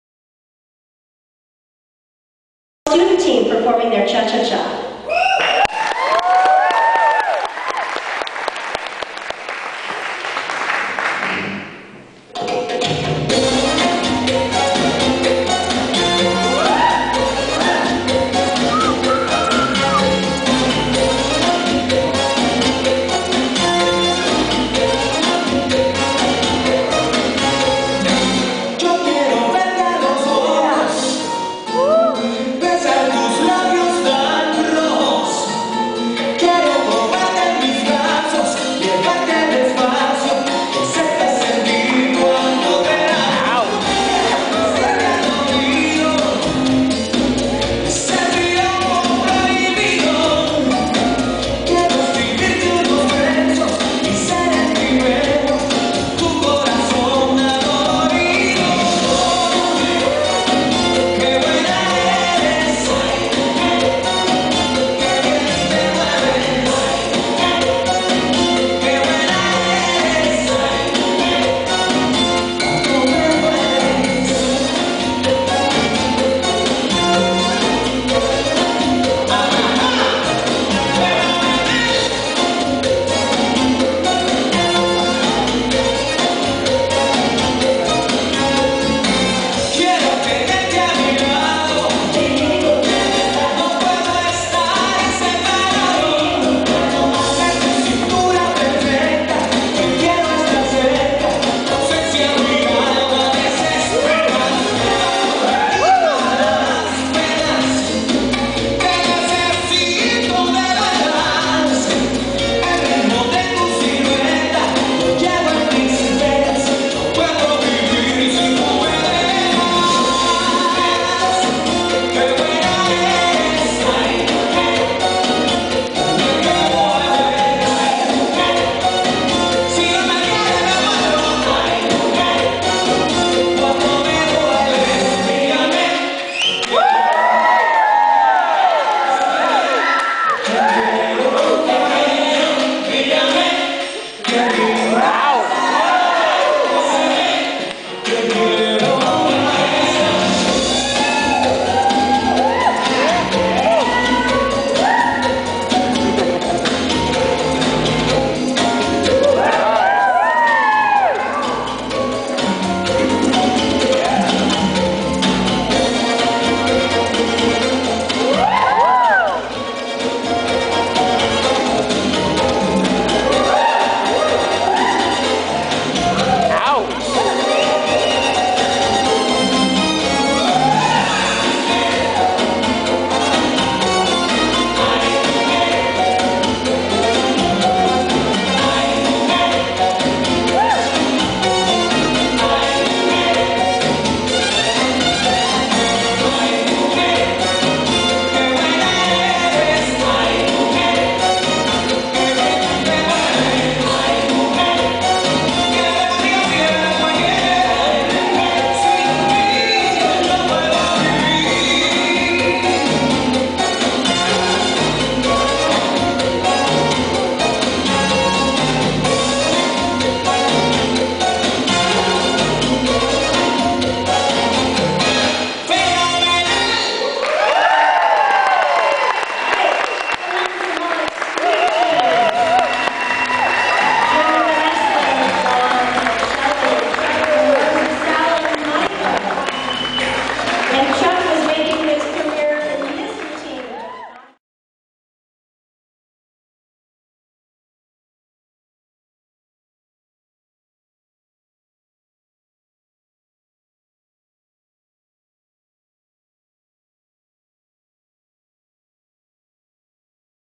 Latin Symbolics Student Team - Cha Cha 12/14/08 at Cheryl burke dance
Latin Symbolics Student Team Performs Cha Cha Cha at Cheryl Burke Dance 12/14/08